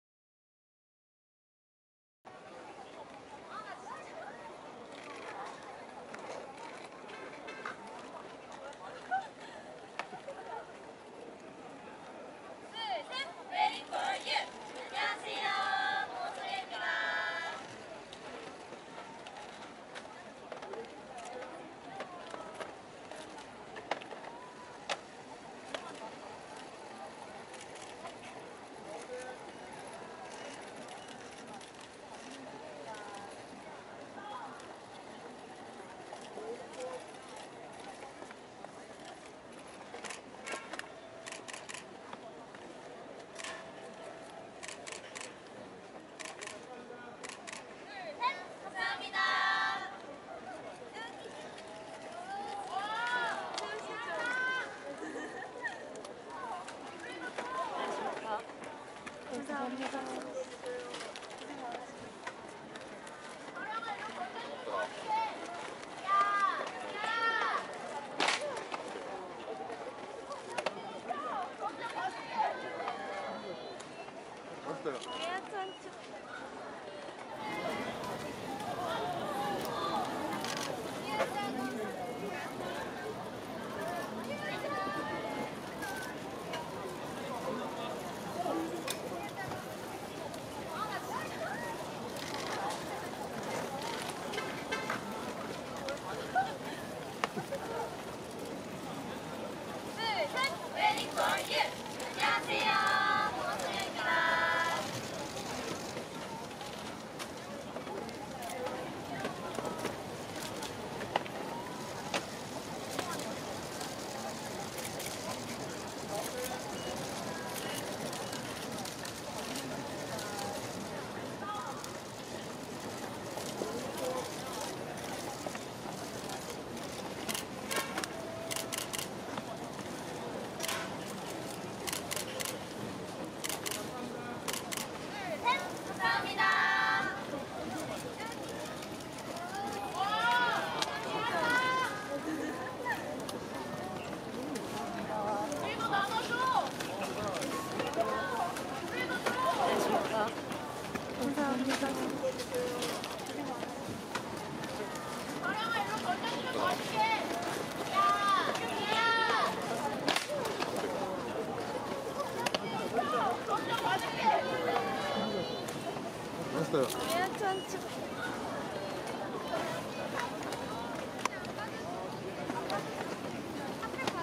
181026 KBS '뮤직뱅크(MUSICBANK)' 공원소녀(GWSN) 할로윈데이 코스튬 출근길
10월 26일 오전 서울시 영등포구 여의도 KBS 신관 공개홀에 KBS ‘뮤직뱅크(MUSICBANK)’ 리허설을 위해 아이돌이 출근을 하고 있다. 할로윈데이를 앞두고 '공원소녀(GWSN)'가 할로윈 코스튬으로 출근하는 출근길 직캠 영상